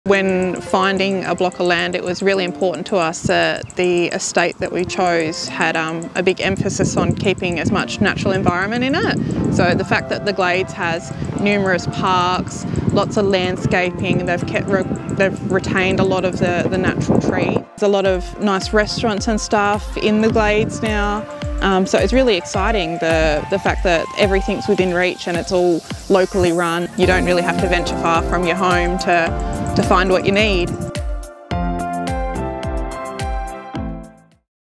Meet The Glades Locals | Roy & Meg 👋🏼
Discover some of the land buyers who have chosen to call The Glades, Byford home.